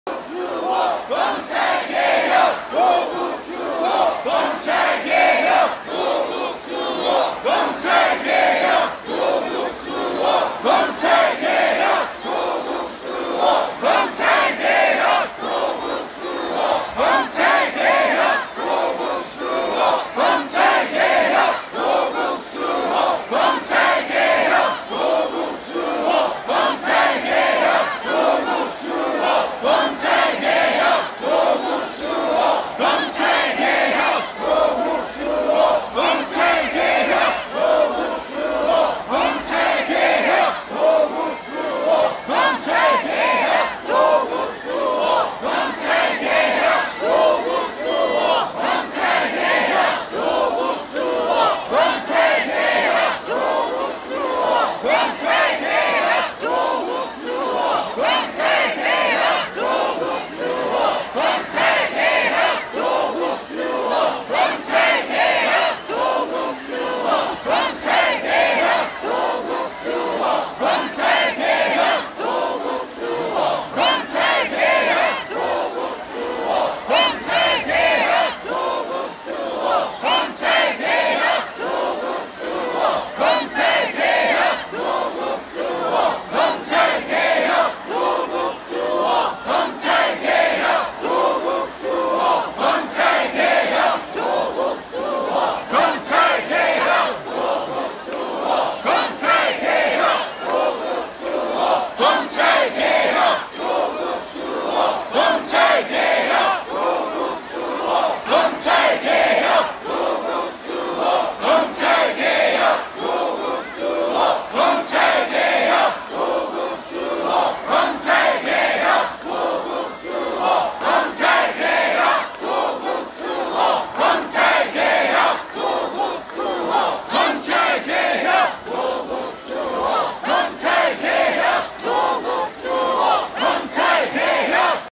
9.28 서초대첩 "조국수호 검찰개혁" 휴대폰 라이트의 위력
9.28 서초대첩 "조국수호 검찰개혁" 휴대폰 라이트의 위력